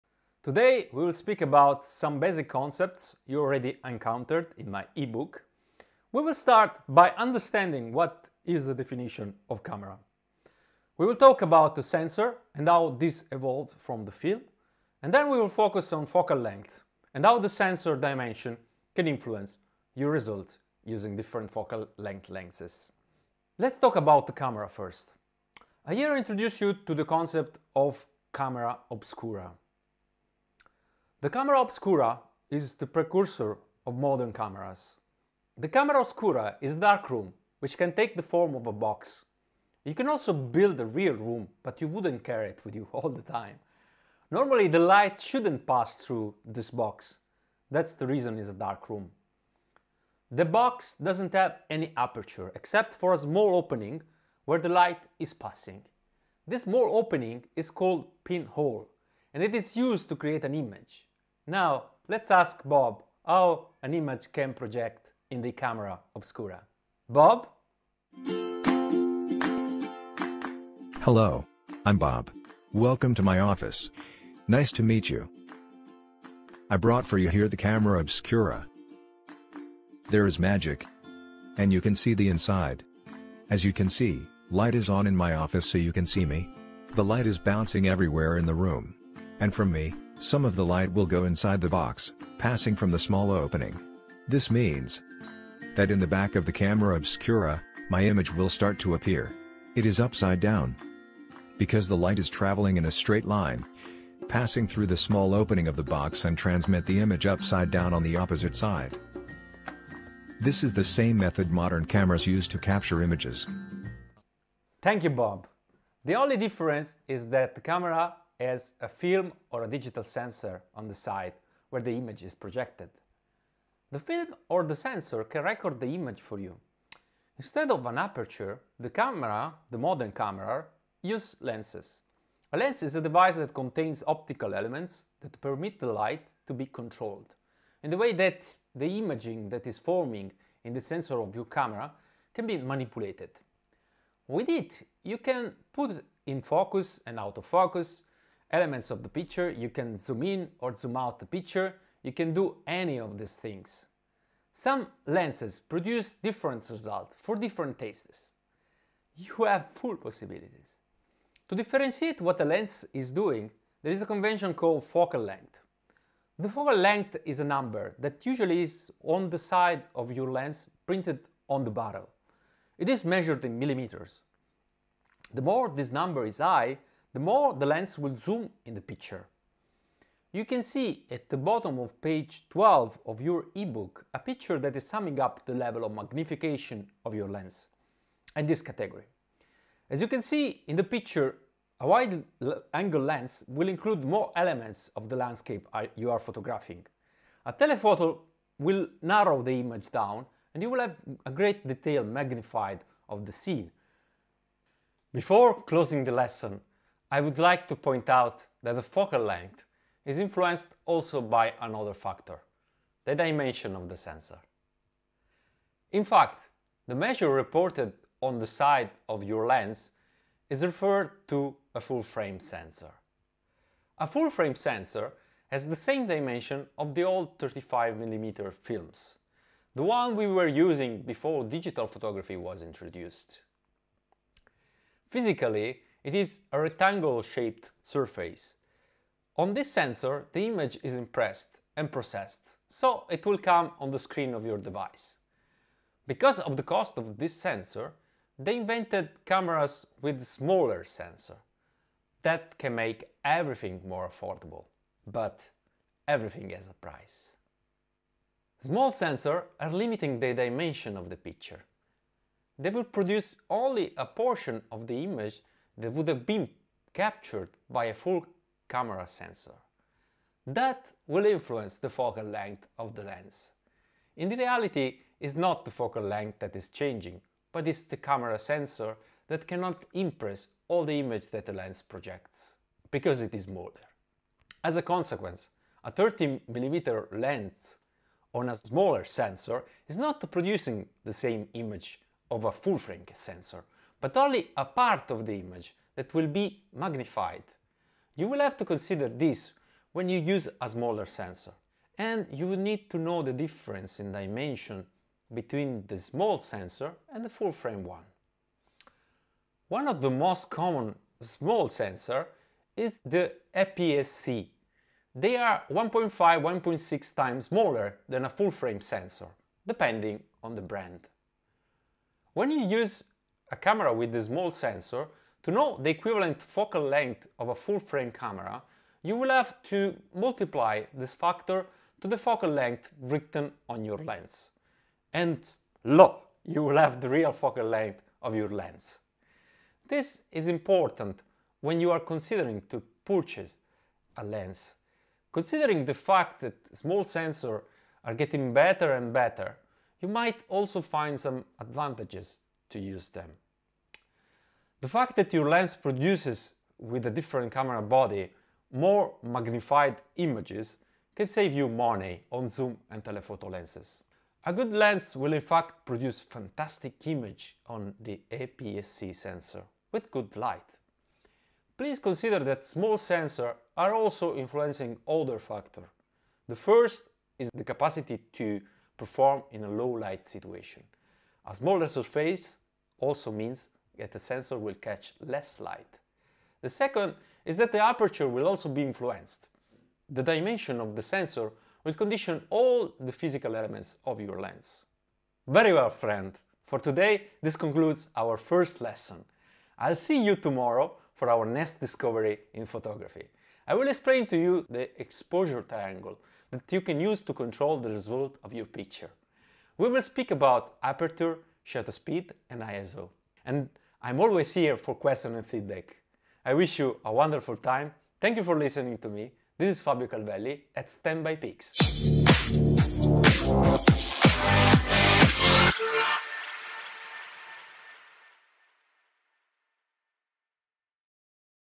Photography Basics in less than 10 Minutes!
The Basics: understand what is a camera, the sensor and the focal length.